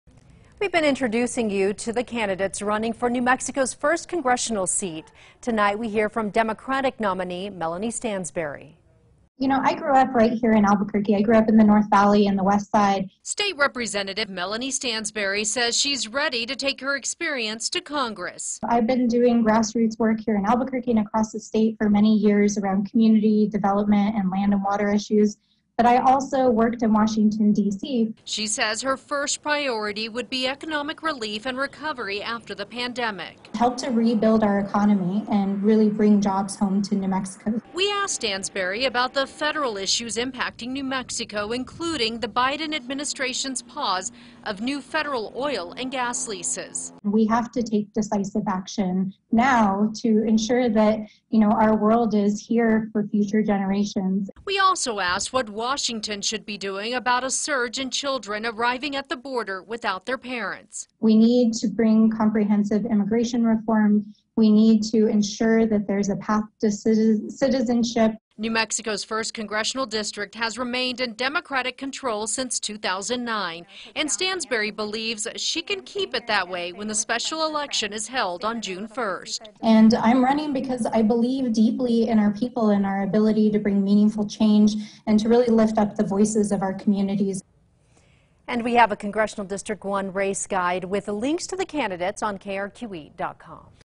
Meet the candidates: CD1 Democratic nominee Rep. Melanie Stansbury
Rep. Stansbury is the Democratic nominee for the special election being held to fill the seat vacated by Deb Haaland.